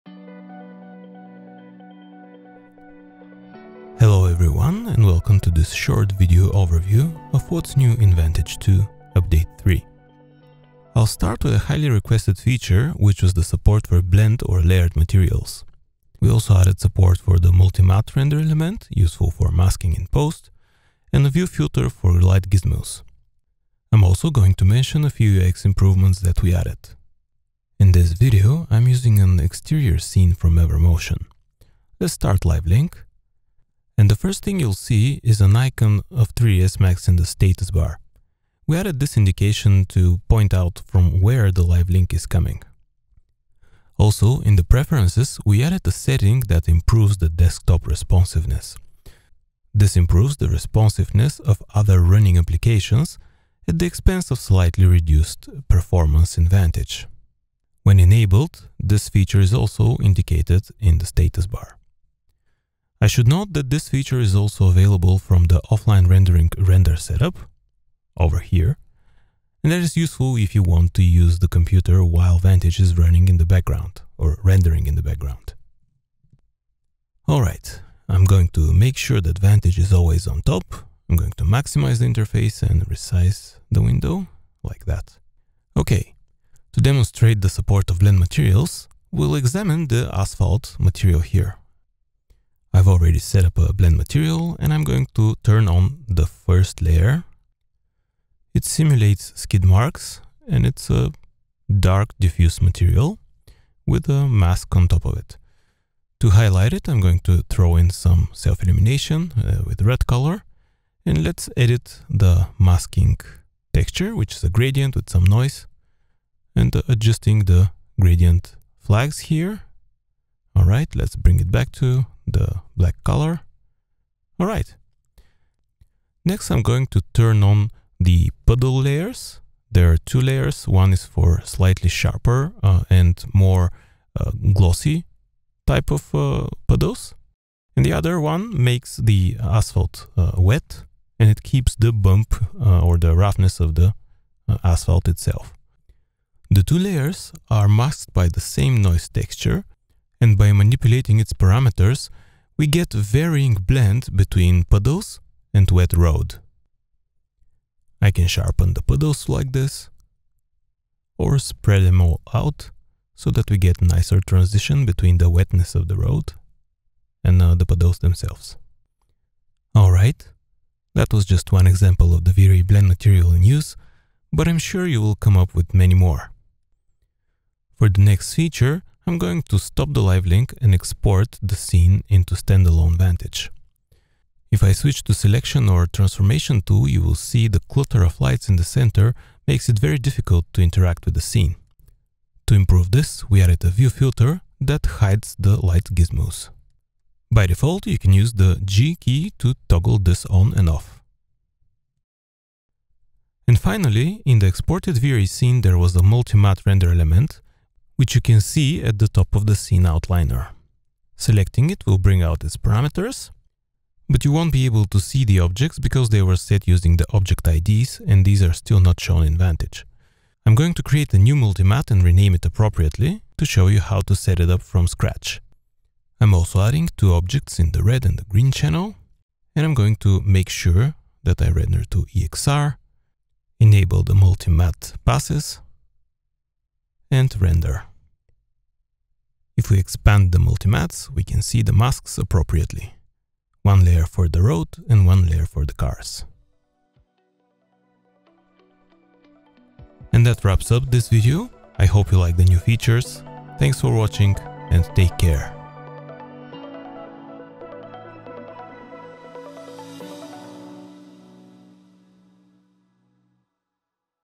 What's New in Chaos Vantage 2, update 3
Chaos Vantage 2, update 3 is out! In this video, Simeon Balabanov, Product Manager for Chaos Vantage explores the latest additions to the product. See how you can create complex materials like wet surfaces with VRayBlendMtl support. Learn how to render pixel perfect masks with Mutli Mattes.

0:00 - Intro
0:35 - Live link host indication
0:46 - Improve desktop responsiveness
1:25 - V-Ray Blend material support
3:05 - View filter for light gizmos
3:27 - Multi Matte render element